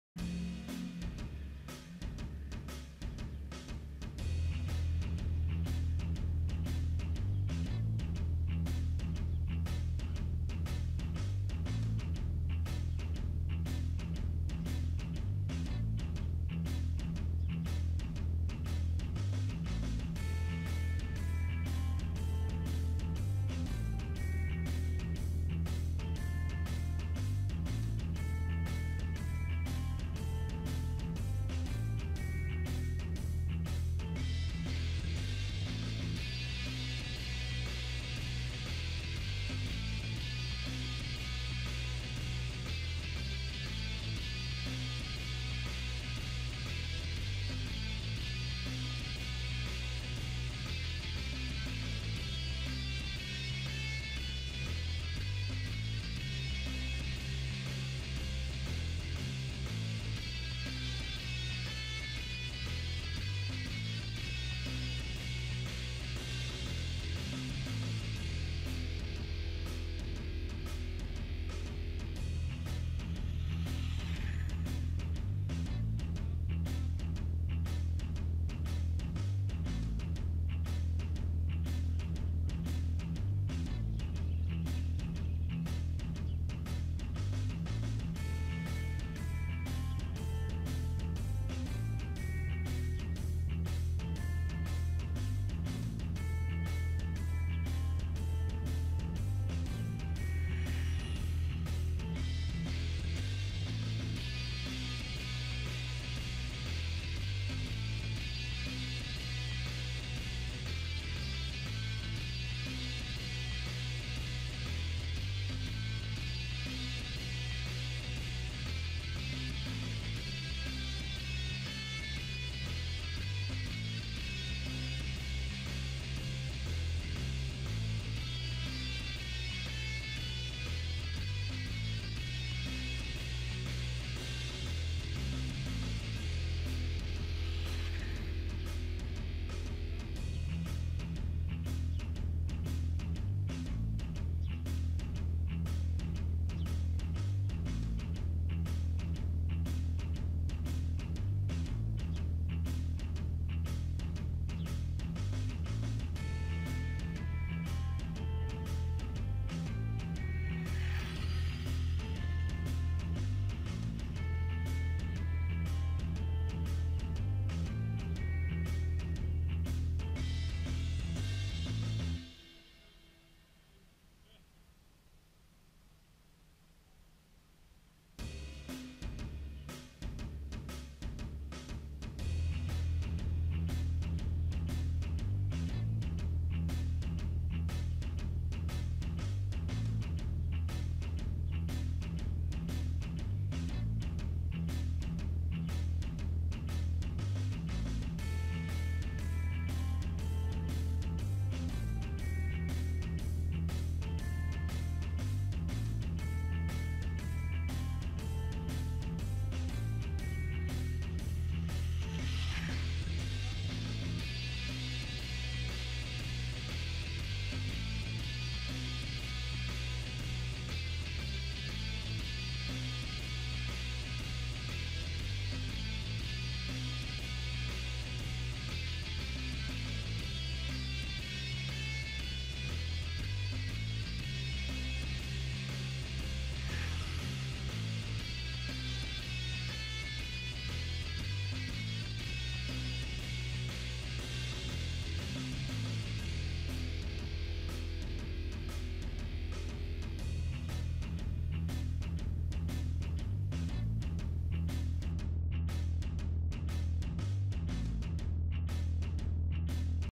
Traxxas Rustler | Driver Under Instruction
Gday guys this week im out testing again and being instructed on the finer points of speed running breakage is not the instructor's responsibility so im told :)

Postal
Deadman RC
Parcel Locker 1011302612
250 Heaths Road
Hoppers Crossing
Vic 3029